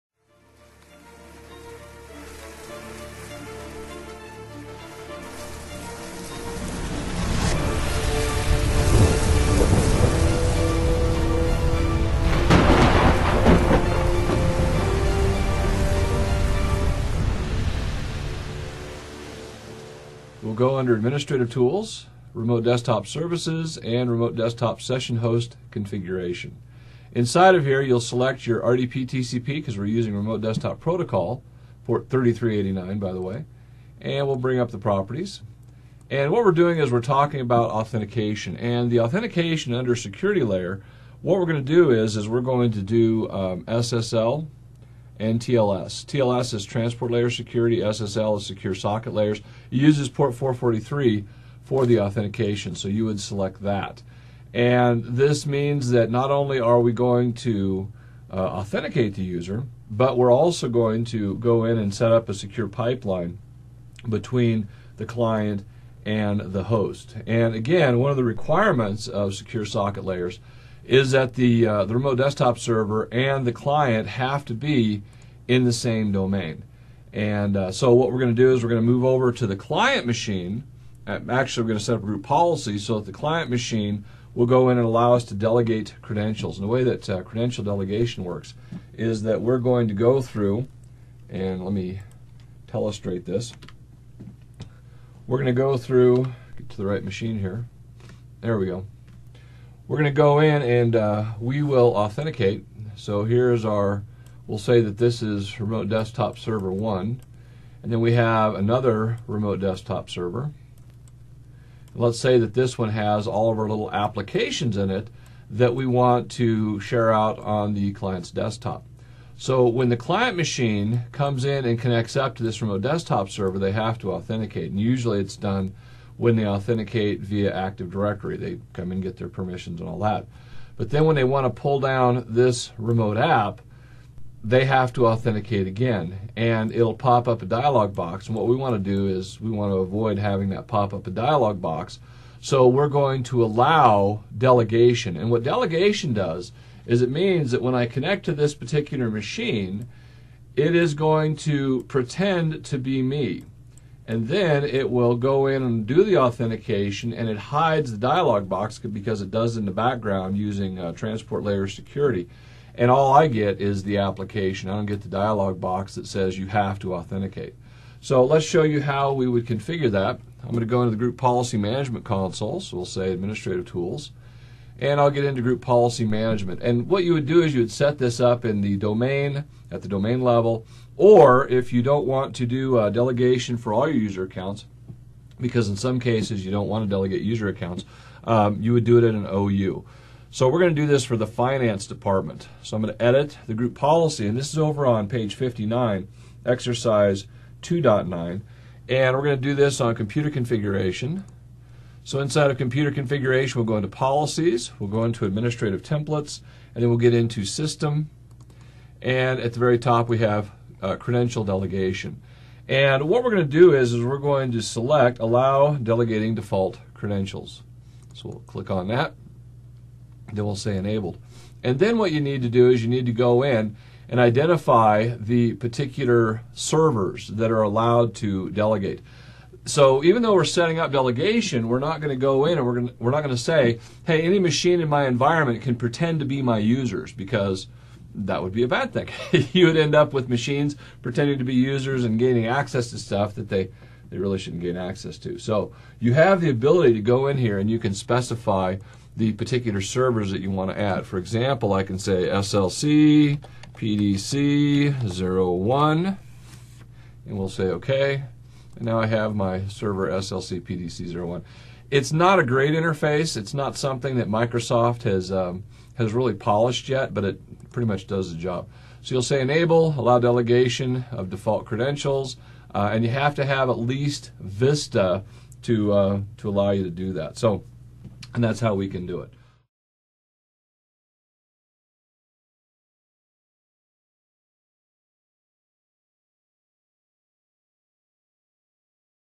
Configuring Single Sign-on and Credential Delegation for Remote Desktop Services
Authentication dialog boxes popping up all the time? Maybe you need to set up Single sign-on (SSO)!

In this excerpt of our Windows Server 2008 R2 Applications Platform class, Senior Technical Instructor Doug Bassett demonstrates how to configure Single Sign-on (SSO) when accessing Remote Desktop servers.

Single Sign-on lets a user authenticate once and then have their credentials passed to other resources. This lets a server "pretend" to be a user so the actual user doesn't have to authenticate over and over when accessing resources.

This makes the user's experience more fluid and makes authentication automatic and transparent. The group policy that Doug configures can be expanded to authenticate with other resources, like SQL server, to enhance user experience and keep the "background stuff" in the background.

This is an example of the real-world, online HD certification training done at Stormwind.com.

We look forward to seeing you in class soon.

StormWind.com